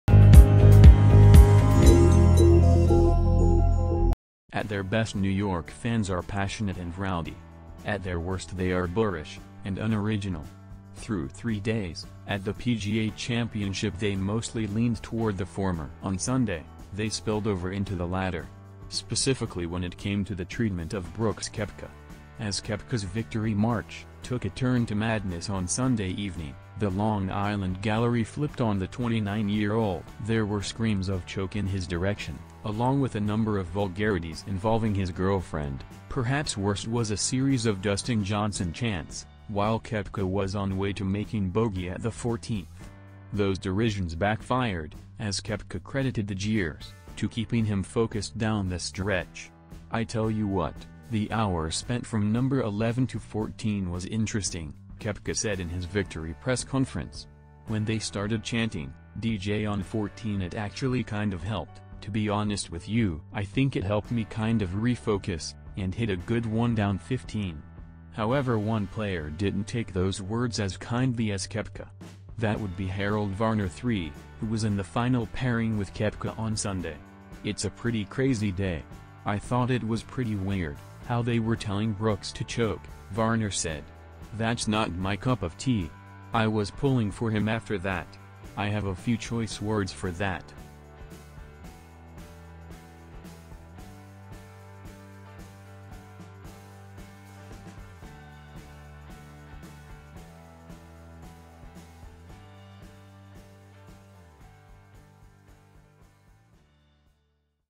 Harold Varner III bashes fans who yelled at Koepka
Harold Varner III bashes fans who yelled at Brooks Koepka: "I have a few choice words for that"

At their best, New York fans are passionate and rowdy. At their worst, they're boorish and unoriginal. Through three days at the PGA Championship, they mostly leaned toward the former. On Sunday, they spilled over into the latter.

Specifically when it came to the treatment of Brooks Koepka. As Koepka's victory march took a turn to madness on Sunday evening, the Long Island gallery flipped on the 29-year-old. There were screams of "CHOKE!" in his direction, along with a number of vulgarities involving his girlfriend. Perhaps worst was a series of Dustin Johnson chants while Koepka was on way to making bogey at the 14th.

Those derisions backfired, as Koepka credited the jeers to keeping him focused down the stretch.

"I tell you what, the hour spent from No. 11 to 14 was interesting," Koepka said in his victory press conference. "When they started chanting, 'DJ' on 14, it actually kind of helped, to be honest with you. I think it helped me kind of refocus and hit a good one down 15."

However, one player didn't take those words as kindly as Koepka. That would be Harold Varner III, who was in the final pairing with Koepka on Sunday.

"It's a pretty crazy day. I thought it was pretty weird how they were telling Brooks to choke," Varner said. "That's not my cup of tea. I was pulling for him after that. I have a few choice words for that."